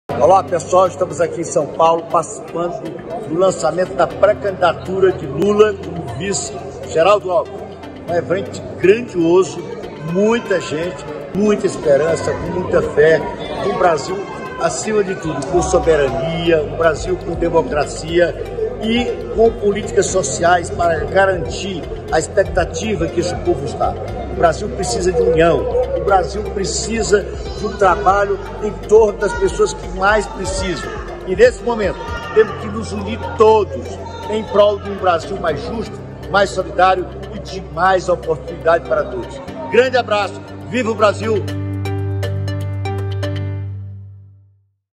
2022 05 07 LANÇAMENTO CANDIDATURA LULA 1
No tbt de hoje, o lançamento da pré-candidatura de Lula à presidência. Estive em São Paulo, ao lado do ex-governador Flávio Dino, de Felipe Camarão e outras lideranças maranhenses para reafirmar sonhos, esperanças e projetos coletivos.

E anota aí: na noite de sexta, dia 13, reuniremos, em São Luís, apoiadores do Time de Lula e Geraldo Alckmin no Maranhão. Será no Master Hall, av. Mário Andreazza e o evento será aberto ao público.